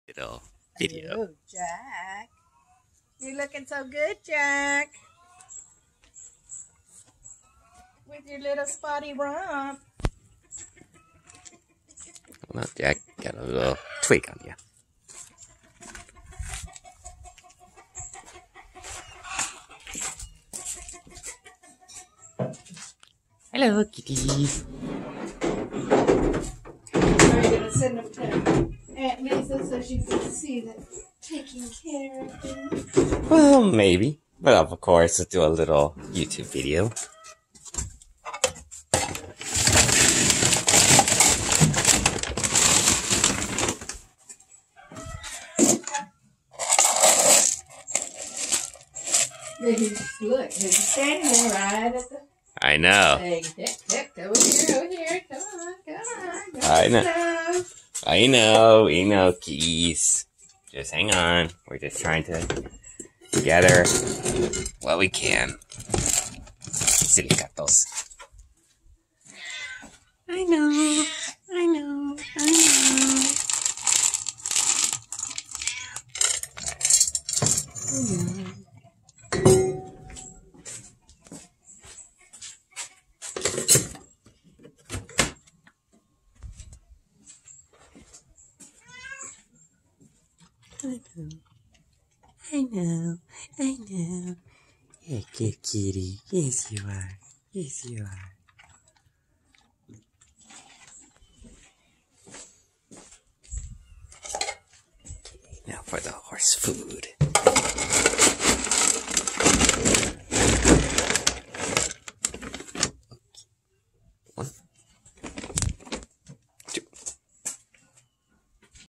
My Mom & I taking care my uncle's animals part 1
feeding my Uncle & Aunt's animals.
miniature horses, chickens, cats, geese & other creatures of the farm.